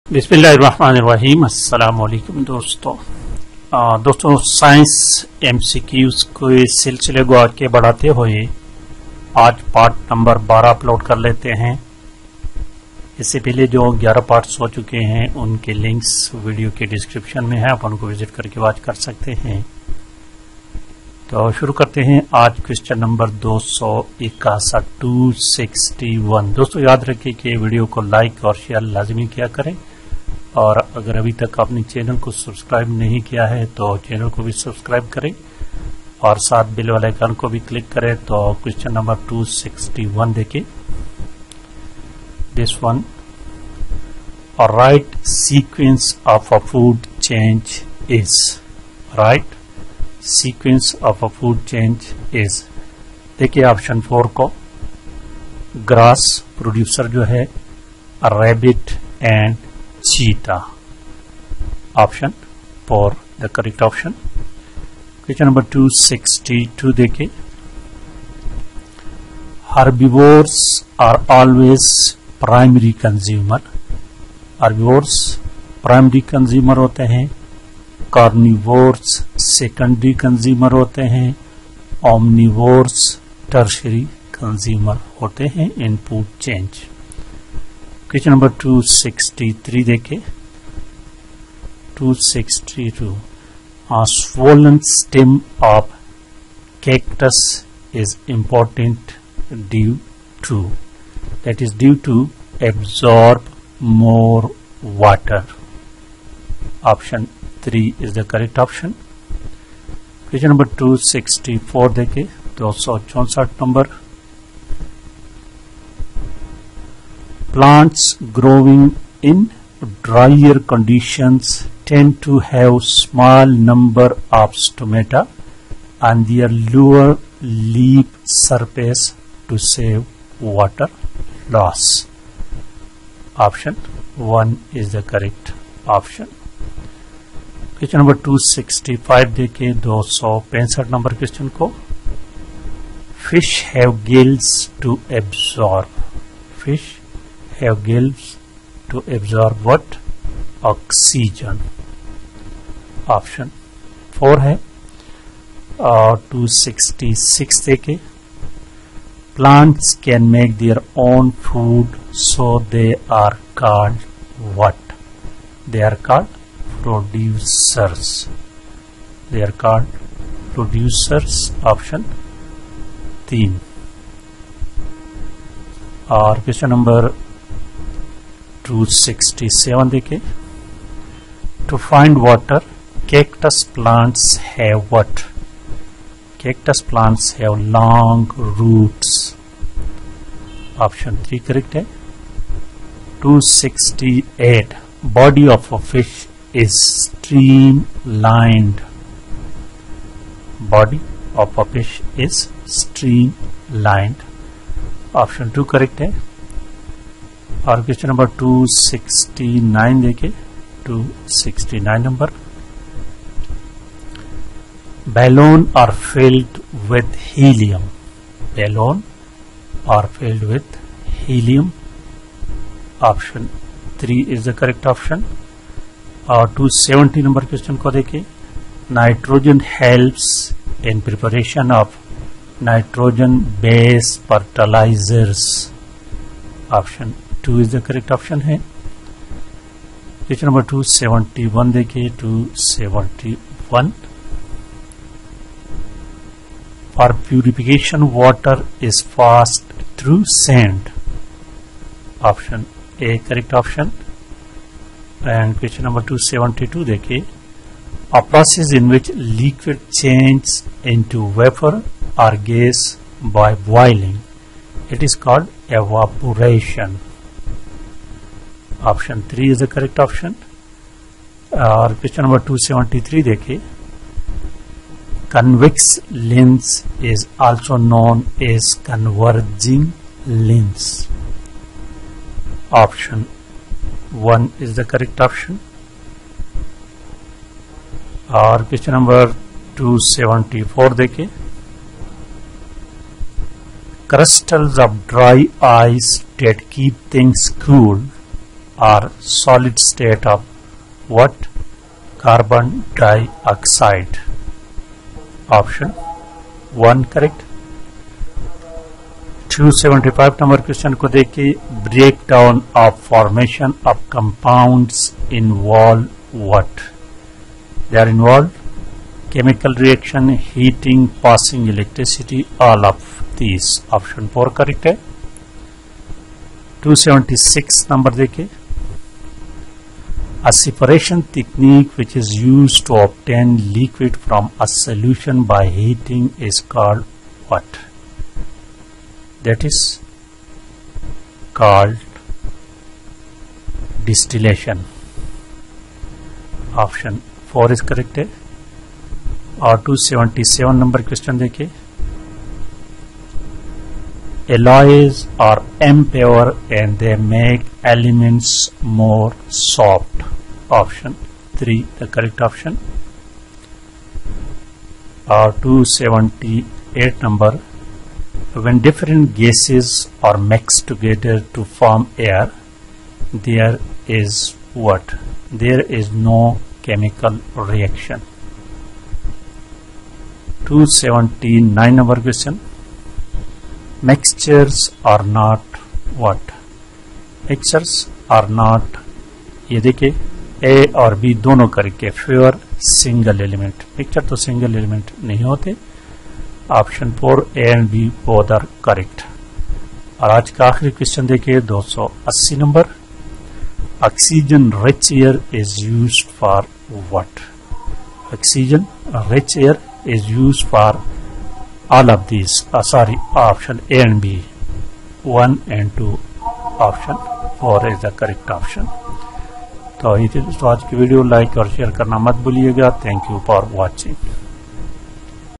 Science MCQs, class 5, 6, 7 and 8 books : PST, JST, JEST, CT, Siba IBA sukar, ETEA, NTS : Part - 12
Dear viewers here i have starting a series of Science MCQs from Books of class 5 to 8: this series is useful for PST, JST, JEST, CT etc candidates.
ETEA, Siba Sukar, NTS etc